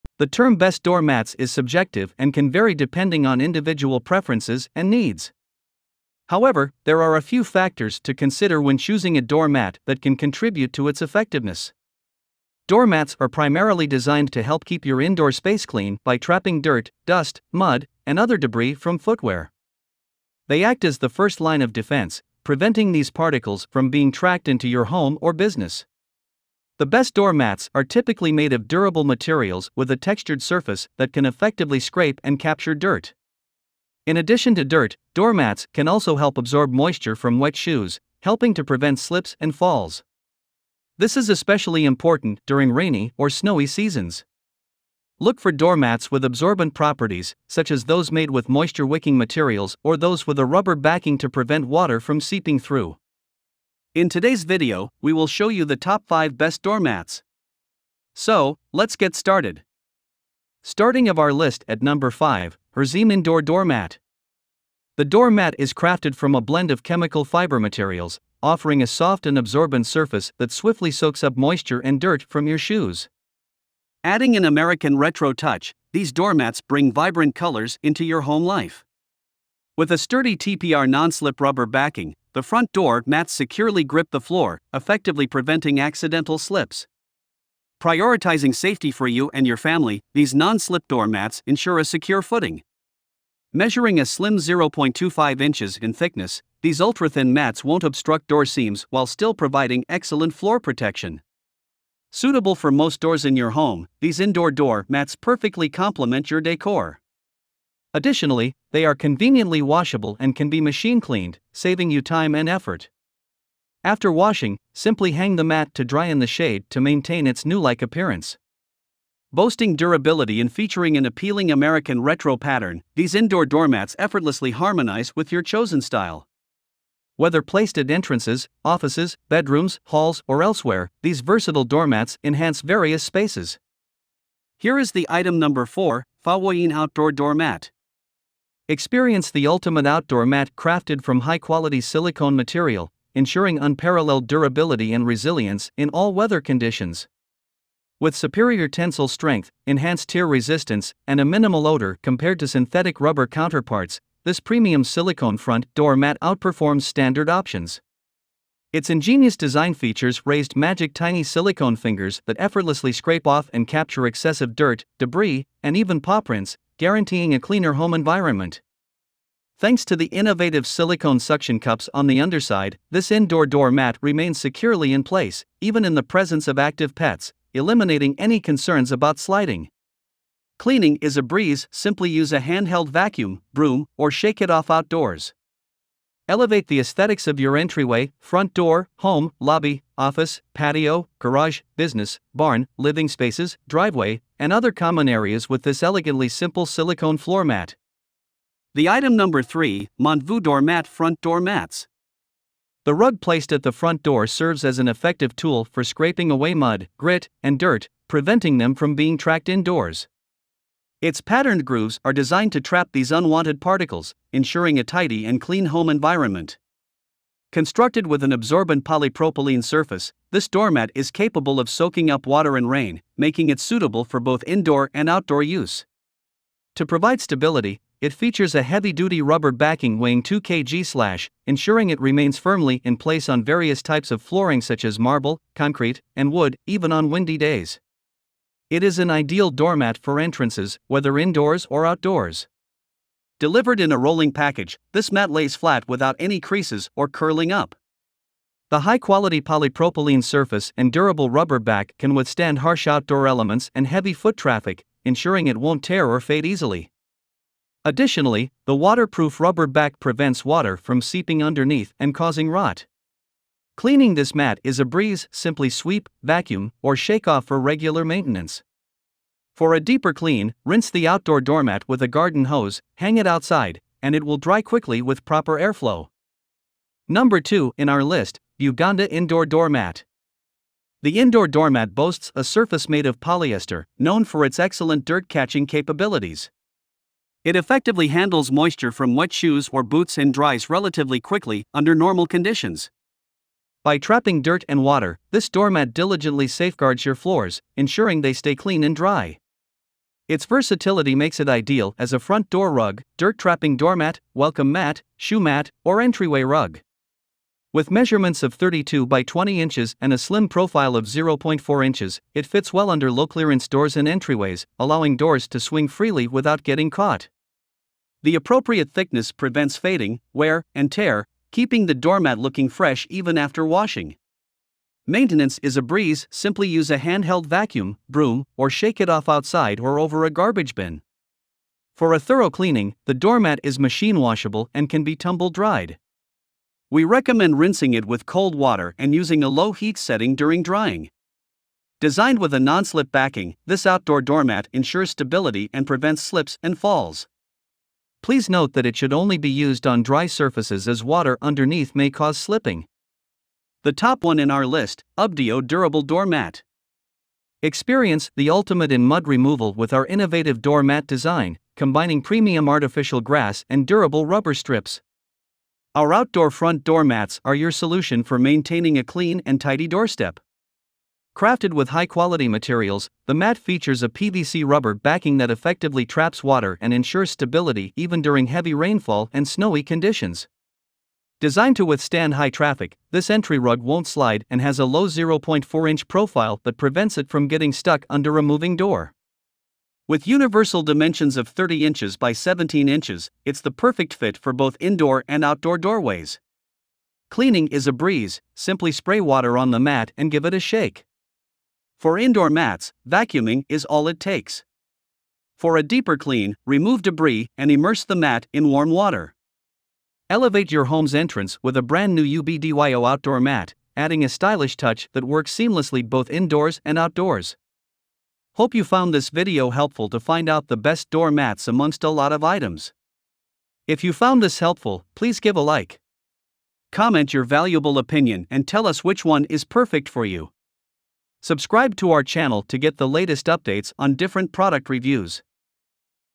Style and Functionality Combined Best Door Mats for Every Home
5. Hrzeem Indoor Door Mat

4. Fowooyeen Outdoor Door Mat

3. MontVoo-Door Mat- Front Door Mats

2. Buganda Indoor Door Mat

1. Ubdyo Durable Door Mat